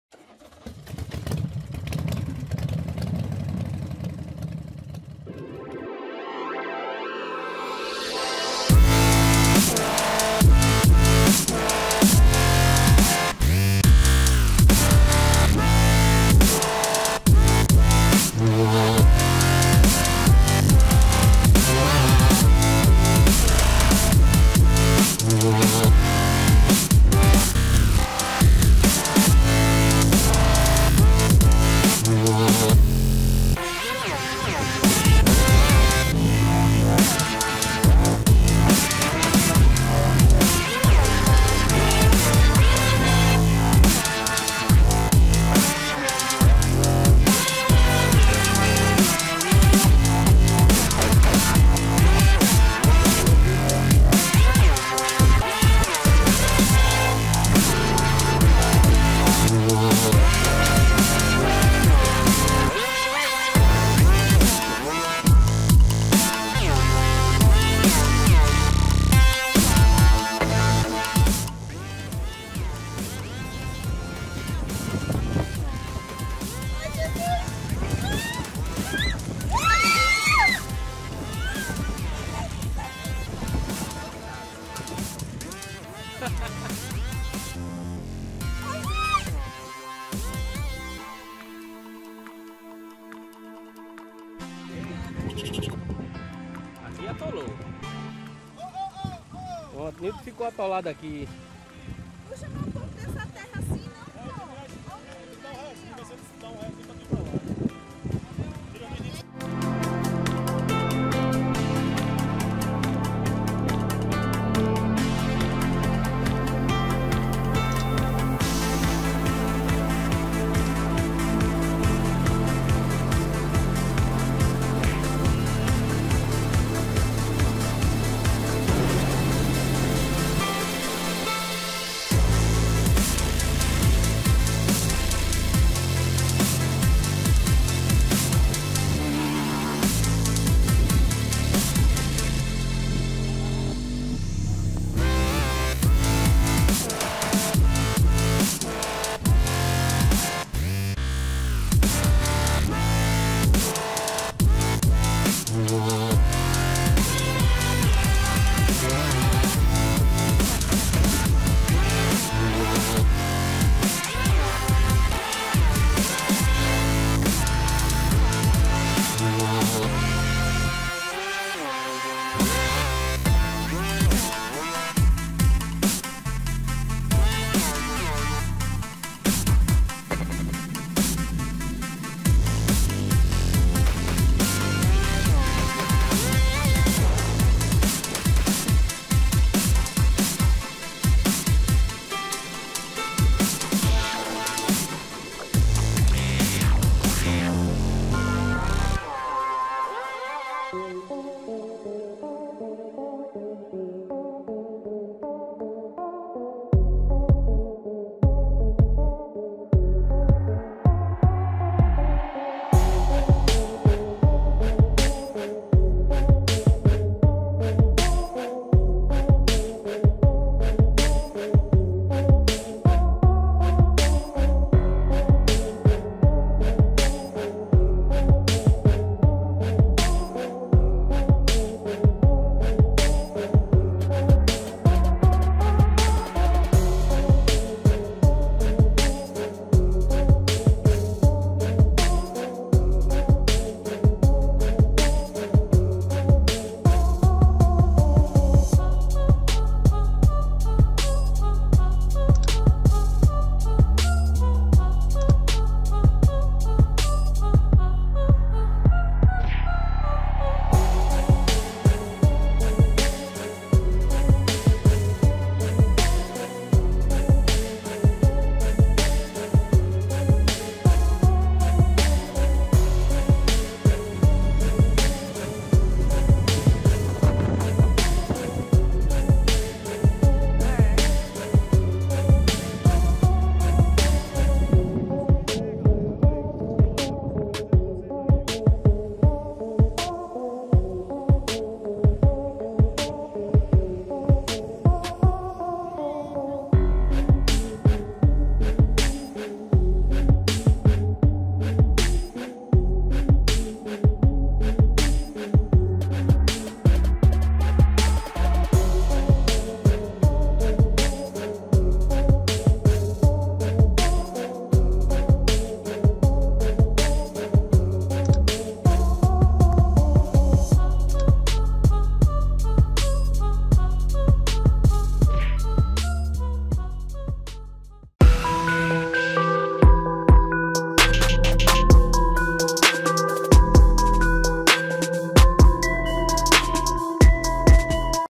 EVENTURA NAS PRAIAS DE SAGI/RN - EQUIPE GBA 4X4
VENHA CONHECER COMIGO AS BELAS PRAIAS DO RIO GRANDE DO NORTE, NESSE DIA FOMOS EM CONHECER A PRAIA DO SAGI.
A Praia do Sagi, localizada no município de Baía Formosa, é a última praia do litoral sul do estado do Rio Grande do Norte, localizada na divisa com o estado da Paraíba.